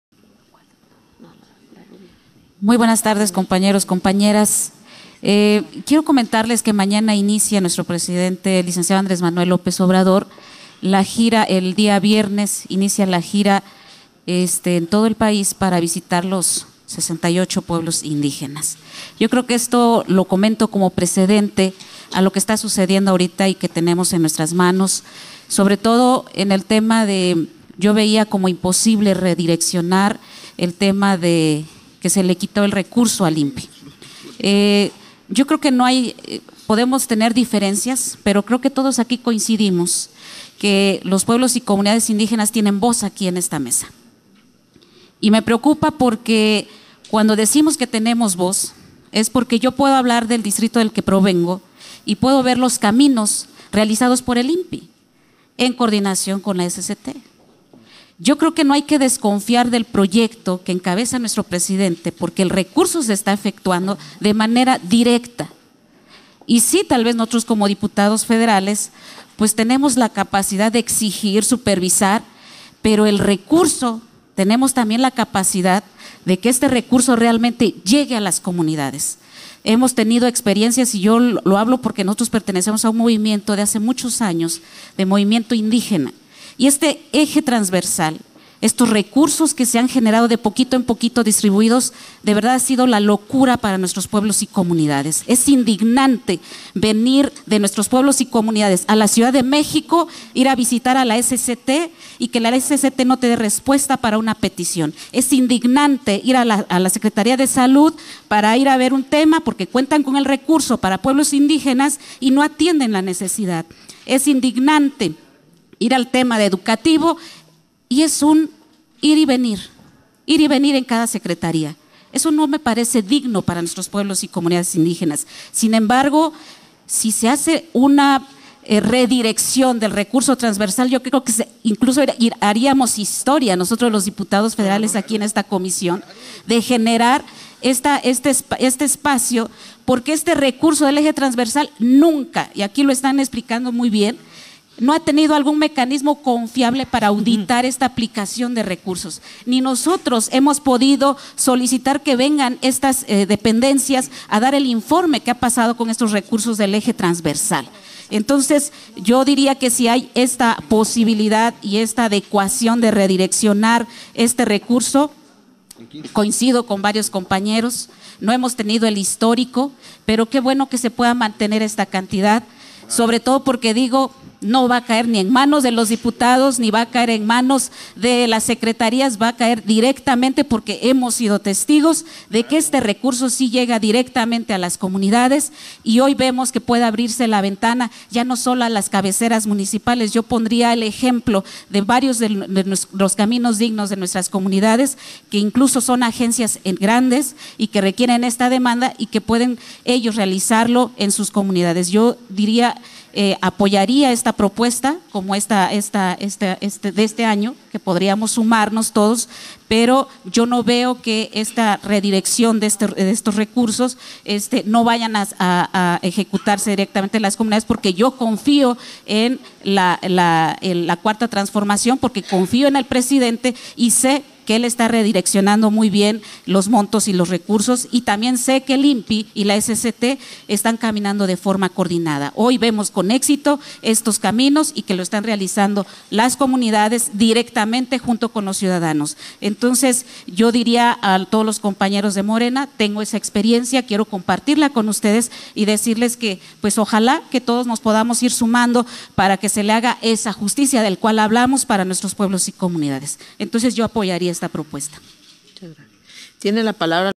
15/10/2019 Participación de la Dip. Beatriz Dominga Pérez López
Participación de la Dip. Beatriz Dominga Pérez López durante la Décima Reunión Ordinaria de la Comisión de Pueblos Indígenas